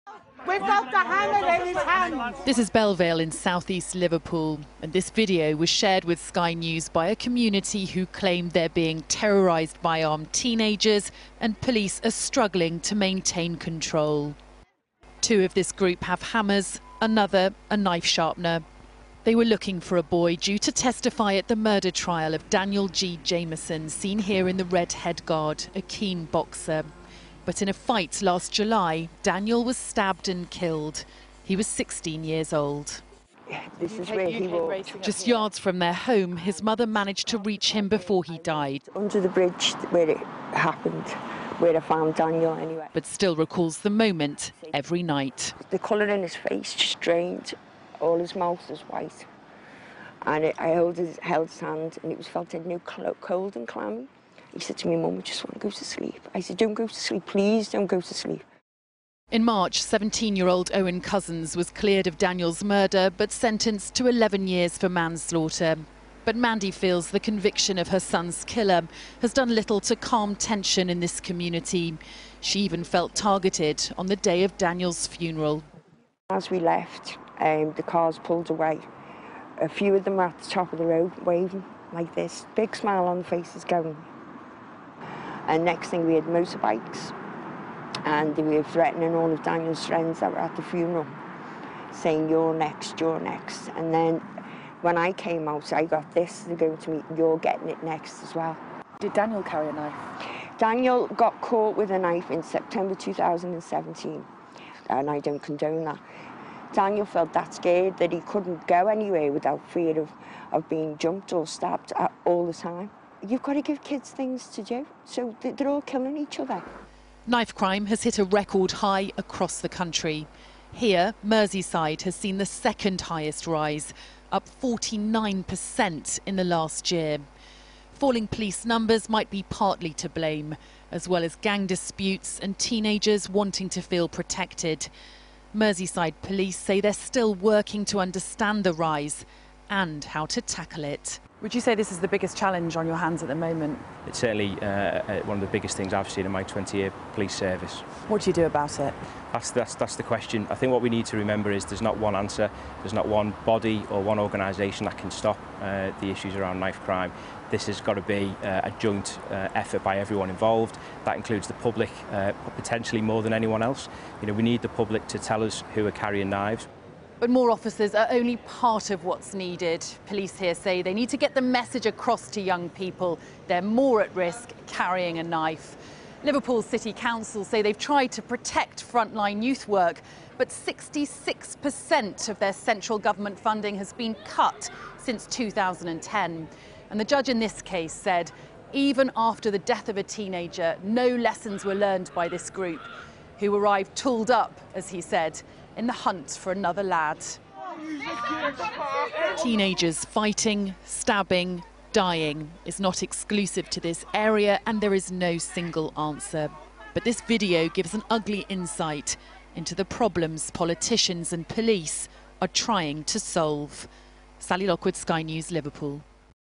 Lawlessness is gripping the streets of Liverpool
The Prime Minister has launched his first major domestic policy - a commitment to recruit 20,000 police officers.

And at almost the same moment that Boris Johnson was speaking, four teenagers were being convicted in a court in Liverpool, in a case that lays bare the lawlessness that police are often confronted with on Britain's streets.

and download our apps